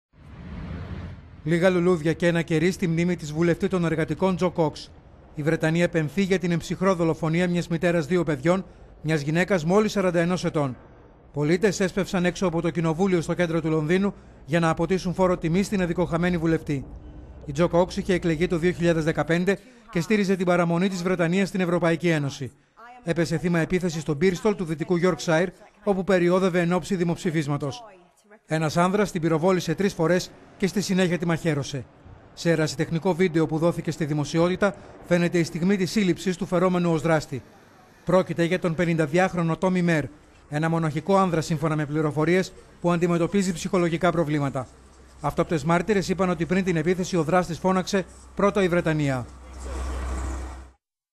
Στο πένθος η Βρετανία μετά την άγρια δολοφονία της βουλευτή των Εργατικών
Με λίγα λουλούδια κι ένα κερί έξω από το βρετανικό κοινοβούλιο, πολίτες θρηνούν για τον άδικο χαμό της βουλευτή των Εργατικών, Τζο Κοξ.

JoCox remembered as tireless campaigner and aid worker TODAY (@TODAYonline) June 17, 2016

Η Βρετανία πενθεί για την εν ψυχρώ δολοφονία μιας μητέρας δύο παιδιών, μιας γυναίκας μόλις 41 ετών.

Η Τζο Κοξ είχε εκλεγεί το 2015 και στήριζε την παραμονή της Βρετανίας στην Ευρωπαϊκή Ένωση.